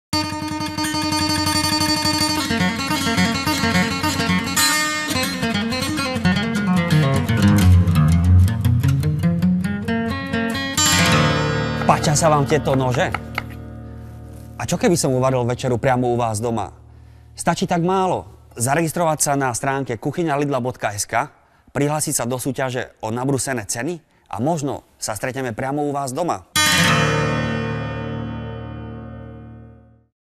Súťaž o nabrúsené ceny - Marcel Ihnačák - Triky kuchynalidla.sk
Nože sú dôležitým nástrojom každého kuchára - chcete sa cítiť ako naozajstný šéfkuchár? Zúčastnite sa súťaže, ktorú nájdete na stránke kuchynalidla.sk a vyhrajte komplet nožov, vďaka ktorým sa z Vás stane profesionál.